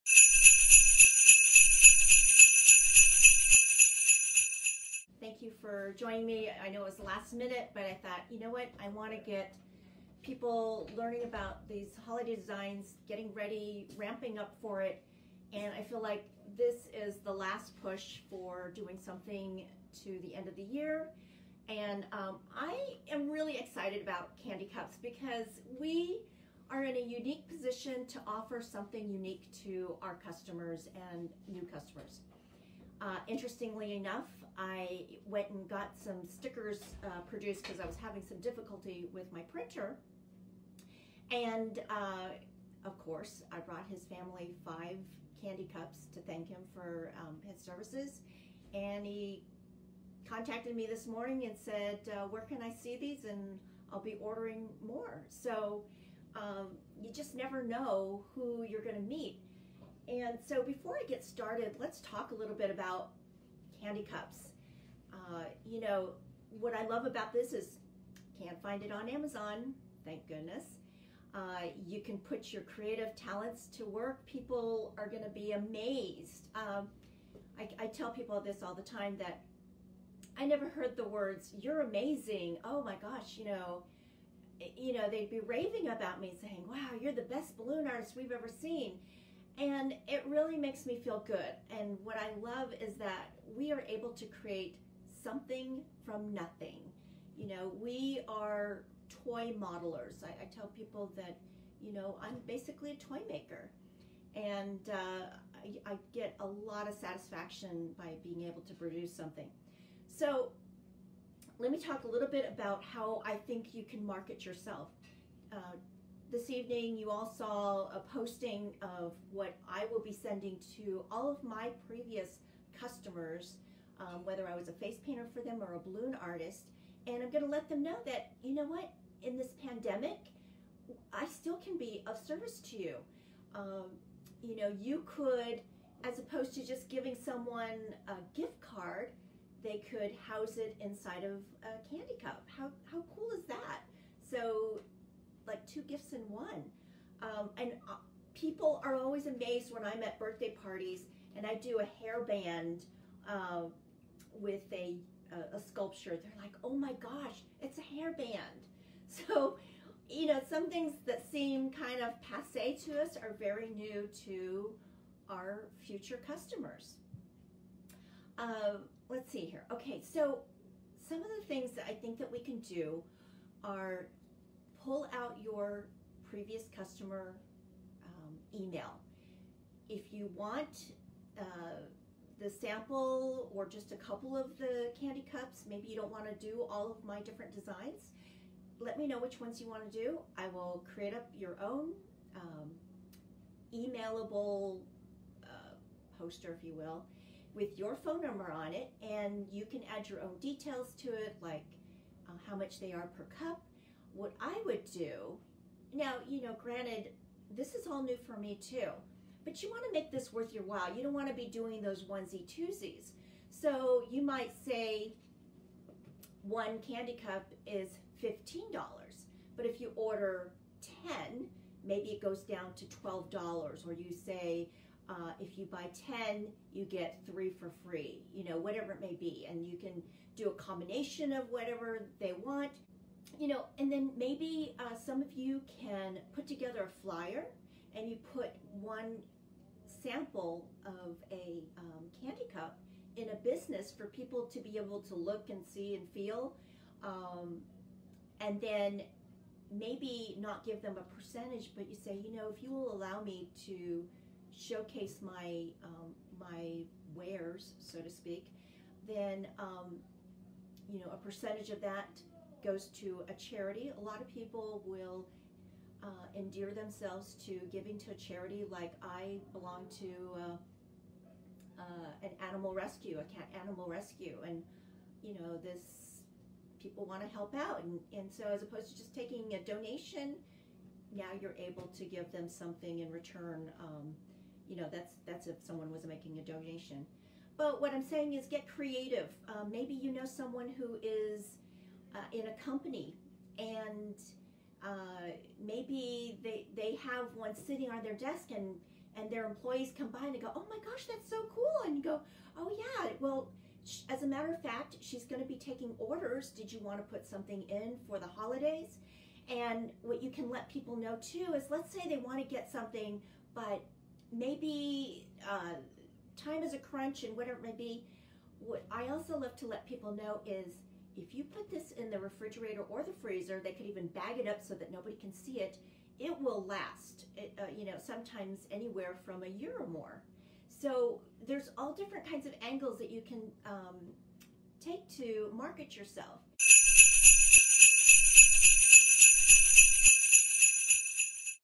Christmas Candy Cup Intro & Marketing
Join Liza as she discusses the concept of candy cups and why they are an amazing upsell at a party or event.

Although this snippet is from a Facebook Live video from during the pandemic, the marketing advice is still very useable far beyond the pandemic so listen to the advice wisely and hopefully you’ll have as much success with candy cups as Liza does.